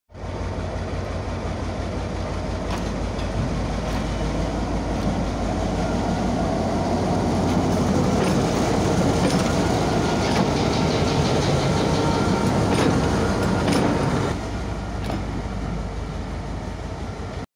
How Diesel Locomotive Engine Works
Locomotives are the best way to travel in the world. These Locomotives Engine moving with the help of using Diesel as fuel. Efficiency of Diesel Locomotives Engine are very good.